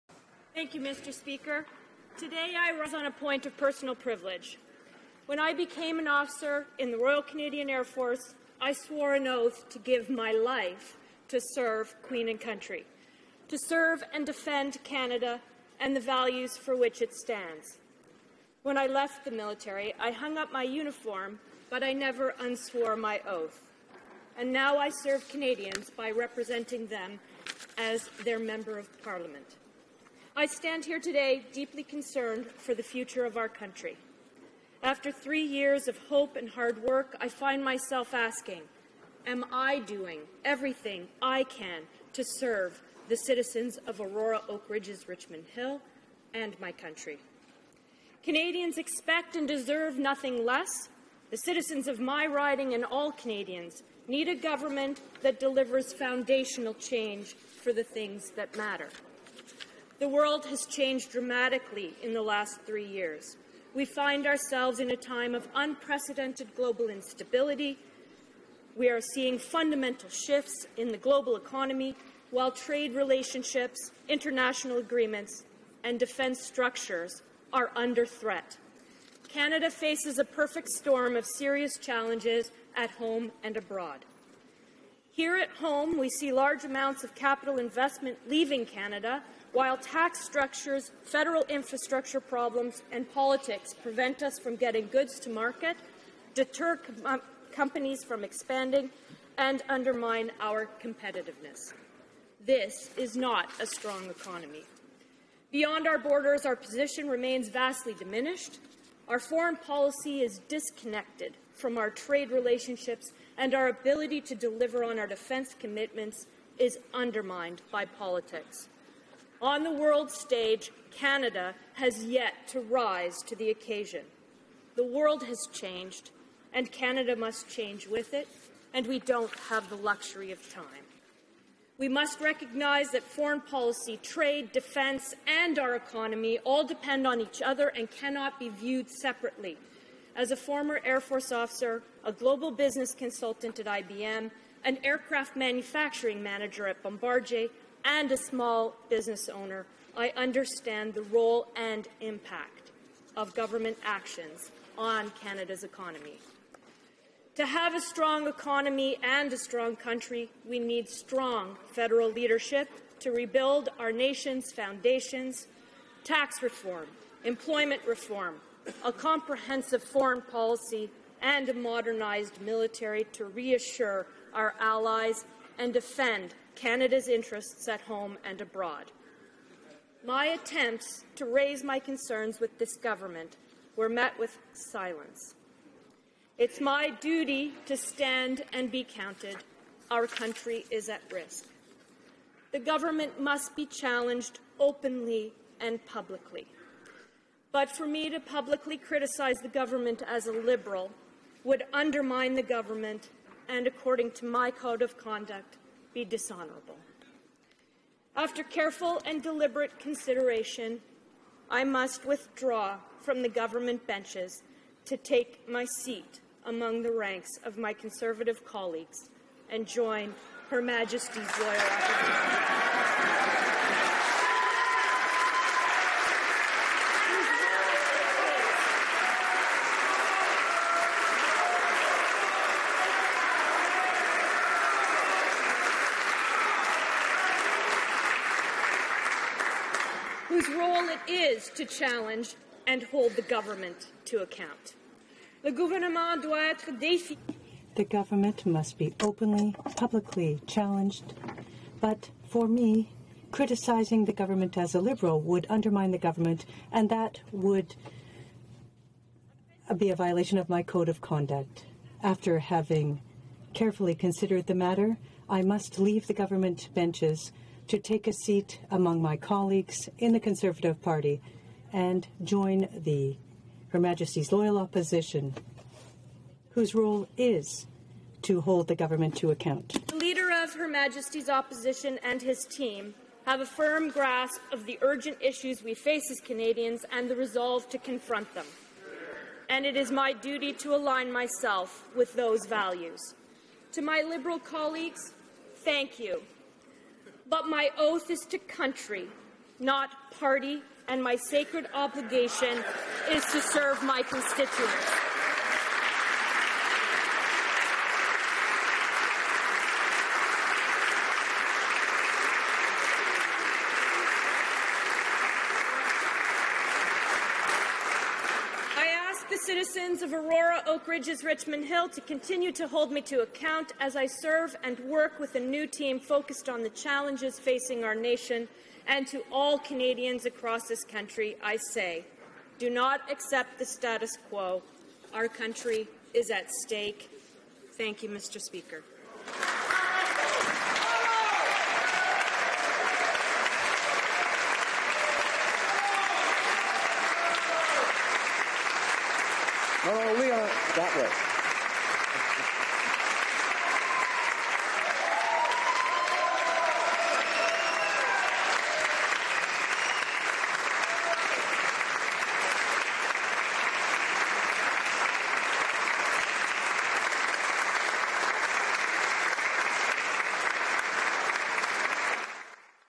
Lib MP Leona Alleslev Crosses The Floor To Join The Cons
Ontario Liberal MP Leona Alleslev leaves the Lib Party because she believes there's a better path for Canada. Conservatives hail her as a hero. Maxime Bernier leaves the Con Party because he believes there's a better path for Canada. Conservatives label him a traitor.